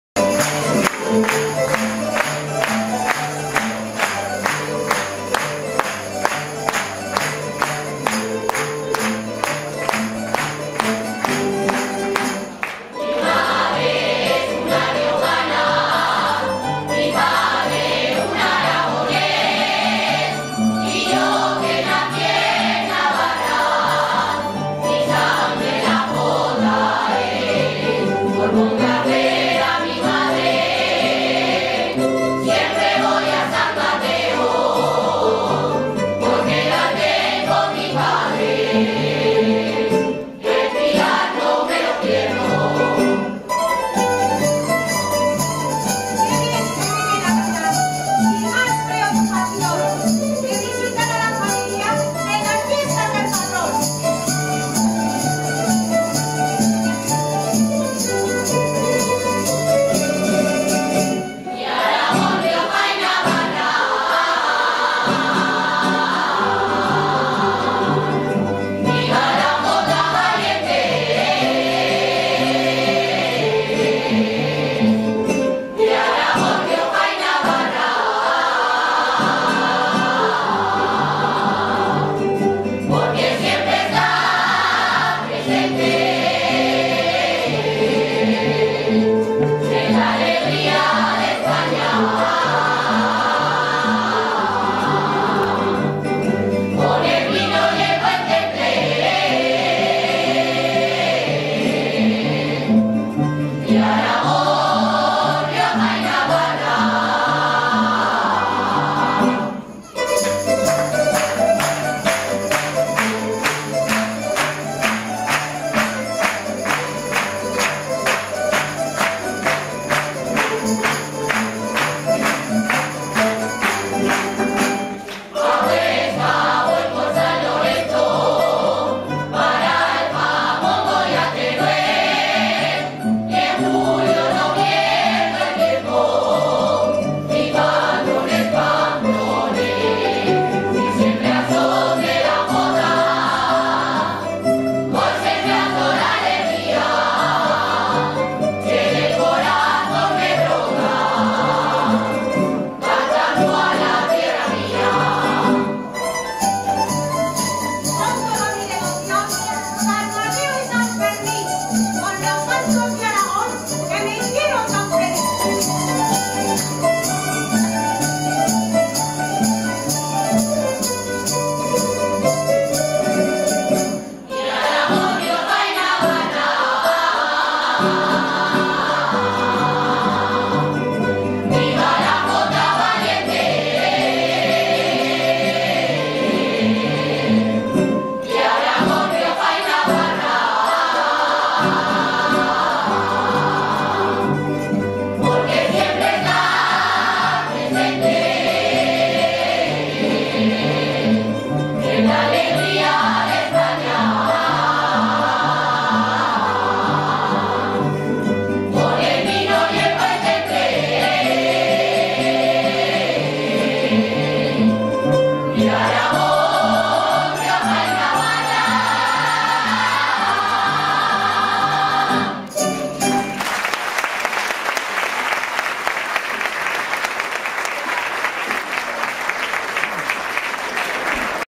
Navarra Rioja y Aragon
Actuacion de la ESCUELA DE JOTAS DE MILAGRO en el Festival benefico que se celebro en Milagro el 3-1-2015